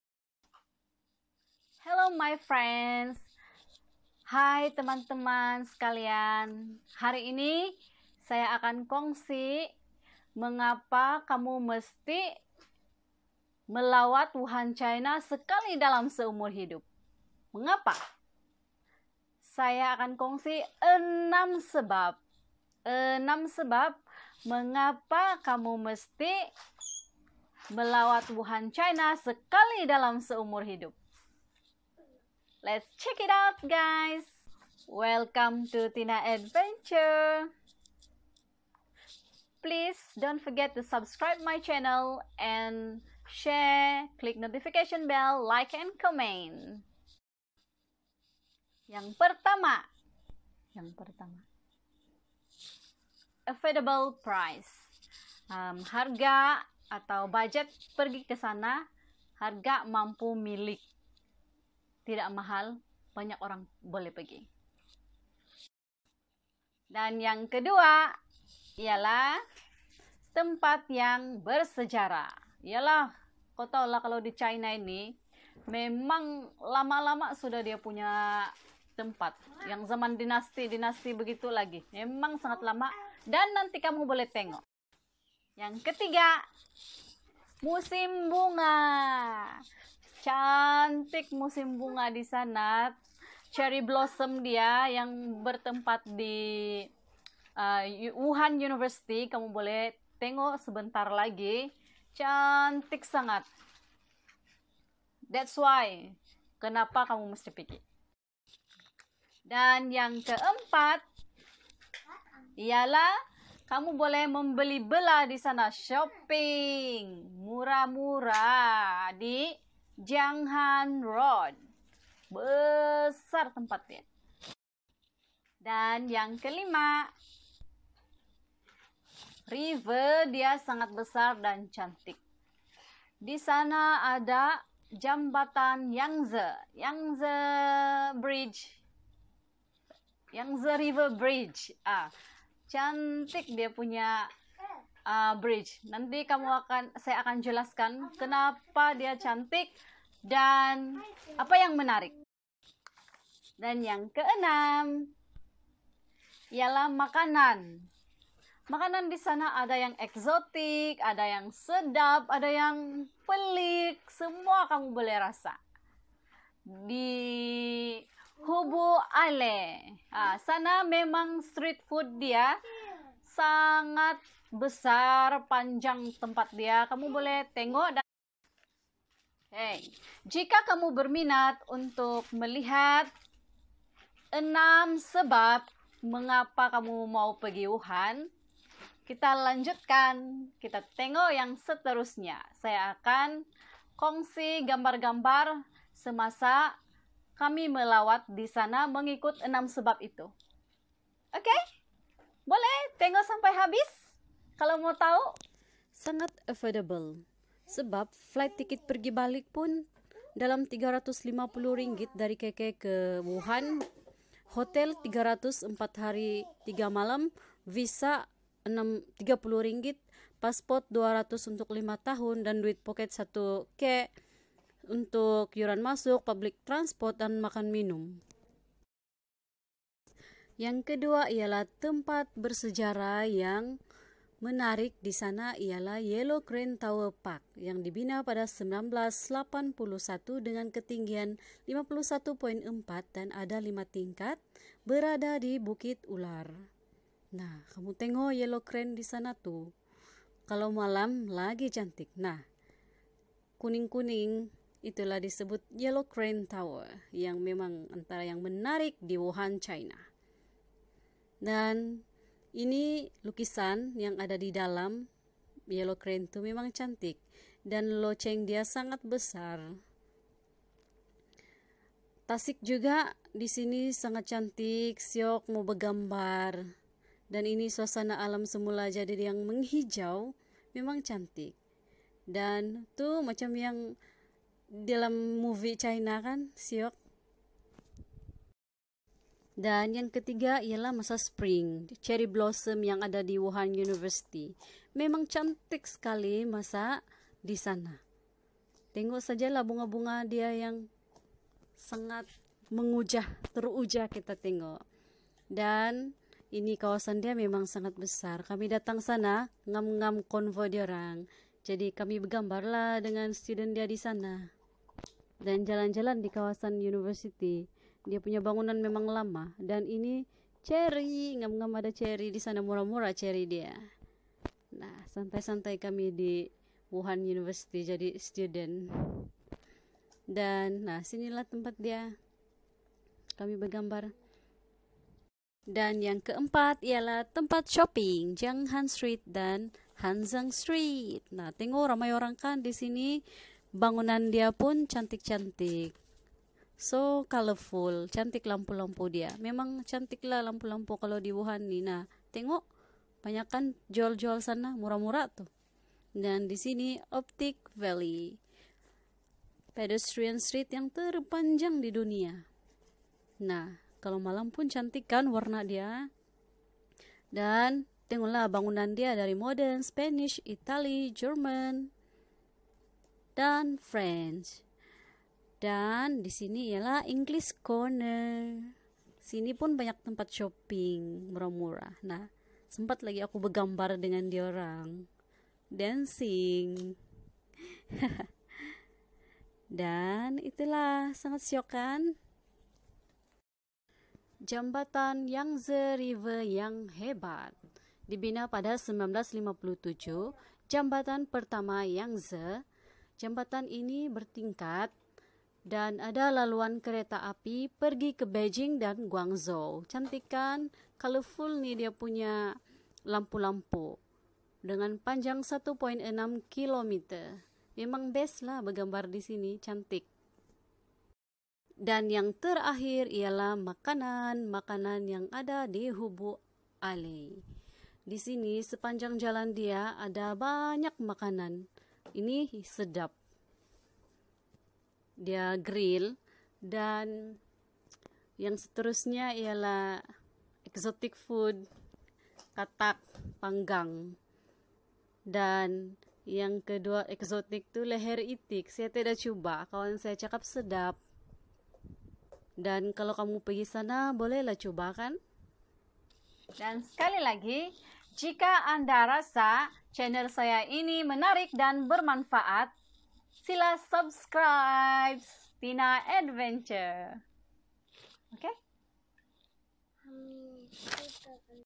AMAZING Place in Wuhan || Mengapa Kita Mesti Melawat Wuhan China Sekali dalam Seumur Hidup?
Disini saya akan kongsi mengapa kita harus melawat Wuhan China Sekali dalam Seumur Hidup. Kita kongsi apa sebabnya.. Why everyone should visit wuhan China once in a lifetime?